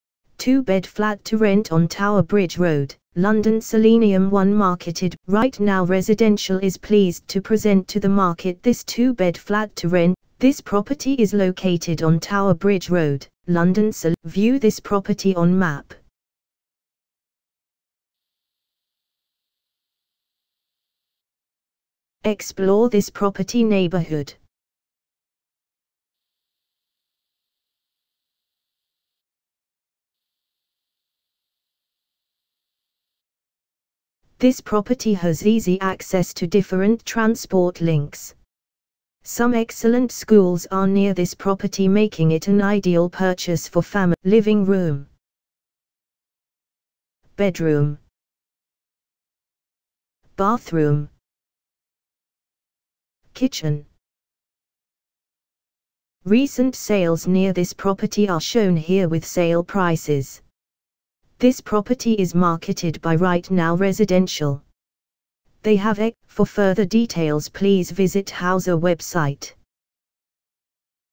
Tower Bridge Road, London SE1 - 2 bed flat to rent
Available Now Large two bed Apartment Fully Furnished to Executive Standard Free wi fi Fully Fitted Kitchen with Washer / Dryer Exposed Brickwork Ample Natural Light 24 Hour Concierge and Security On site Viewing Highly Recommended Stunning two bed double bedroom apartment 5 min walk to London Bridge Station. Fully furnished to executive standard. Wooden floors throughout and a modern fully fitted kitchen with washer/dryer. Exposed brickwork. 24 hour concierge, security and CCTV.